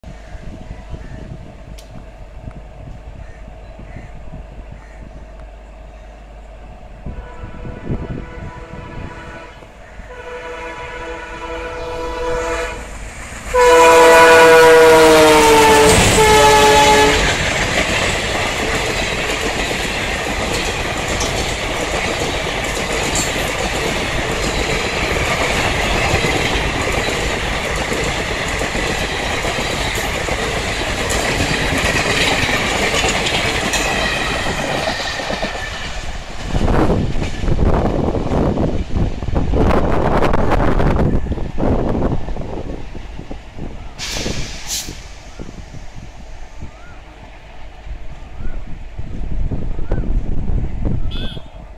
BNDM siblings rampage through Kalupadaghat hauling the Amritsar Visakhapatnam Hirakud Express
Date: 1-Jun-2015